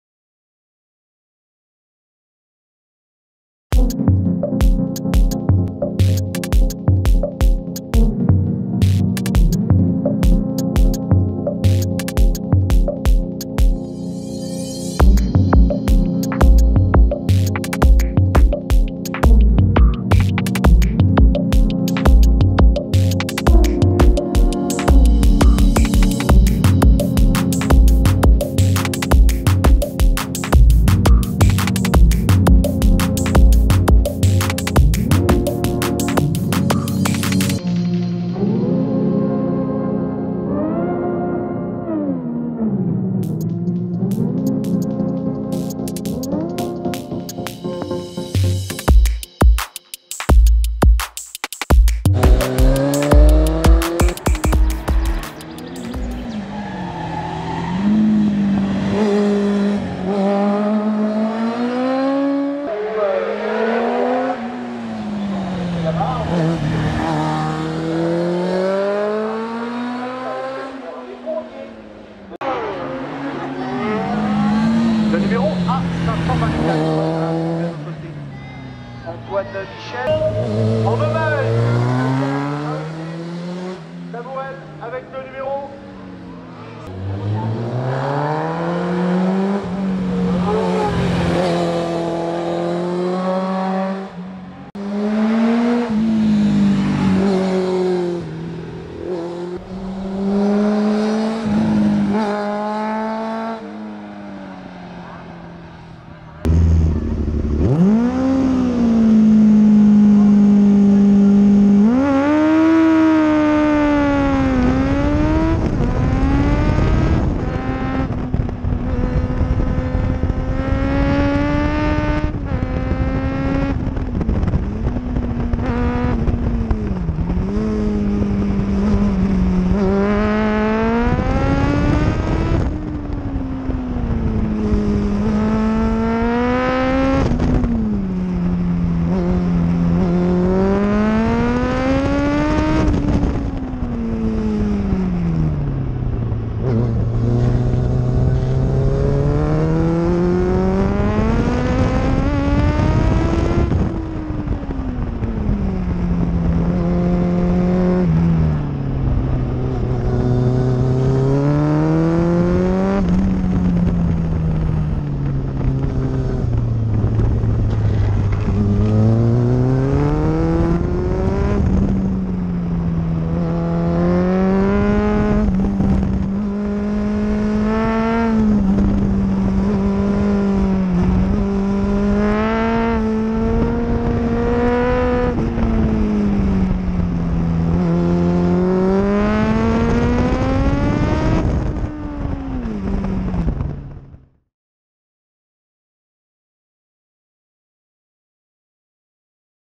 Championnat de France de course de cote 2022 Chanaz
2ème manche du championnat de France de course de cote 2022 en catégorie classique solo

Résultat du Week-end: 3ème